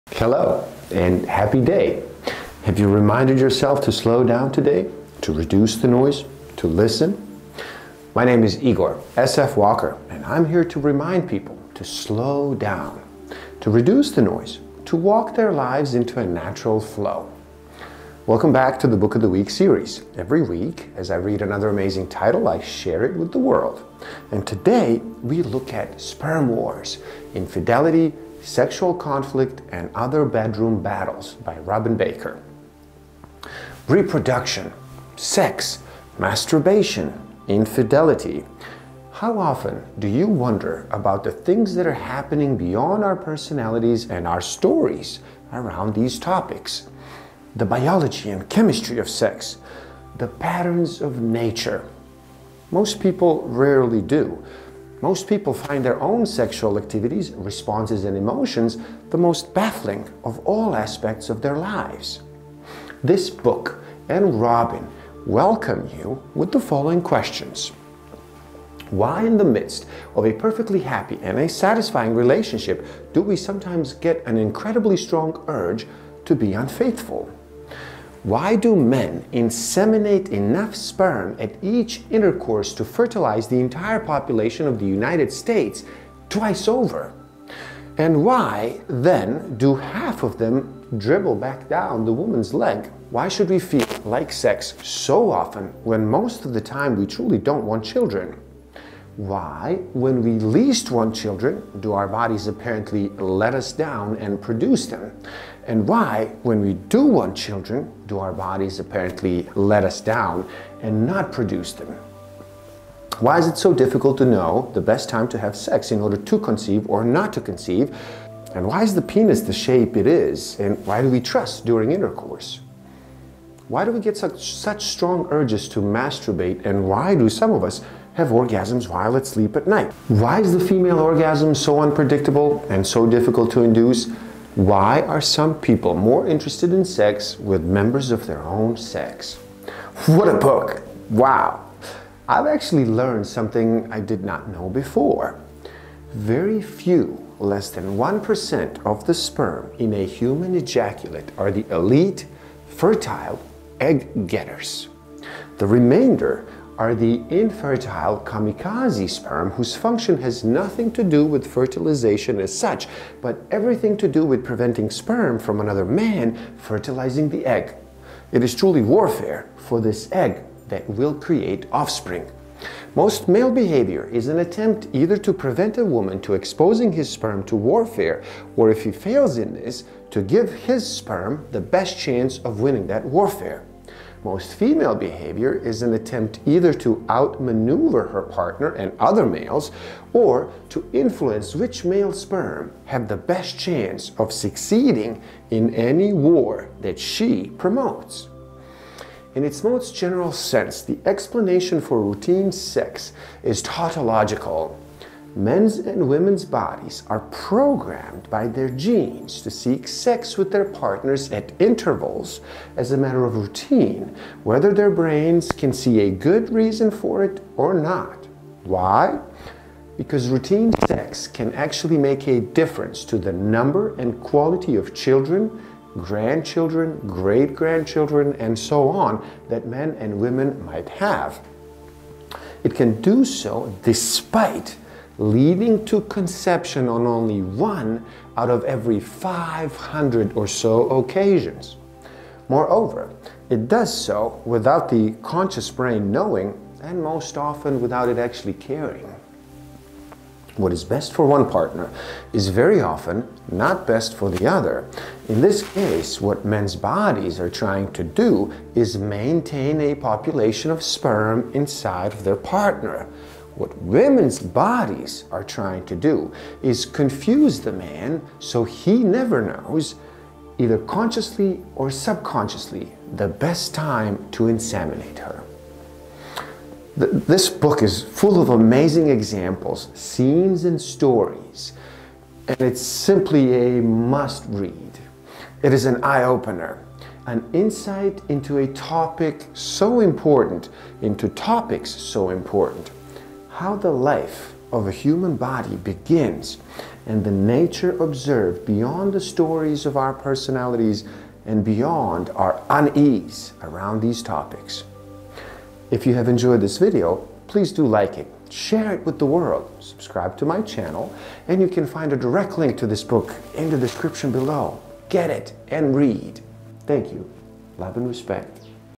Sperm Wars: Infidelity, Sexual Conflict, and Other Bedroom Battles by Robin Baker Book Review
Sperm Wars: Infidelity, Sexual Conflict, and Other Bedroom Battles by Robin Baker
Sex isn't as complicated as we make it. In Sperm Wars, evolutionary biologist Robin Baker argues that every question about human sexuality can be explained by one simple thing: sperm warfare. In the interest of promoting competition between sperm to fertilize the same egg, evolution has built men to conquer and monopolize women while women are built to seek the best genetic input on offer from potential sexual partners.

GET IT. READ :)

Human Needs Psychology + Emotional Intelligence + Universal Laws of Nature = MASTER OF LIFE AWARENESS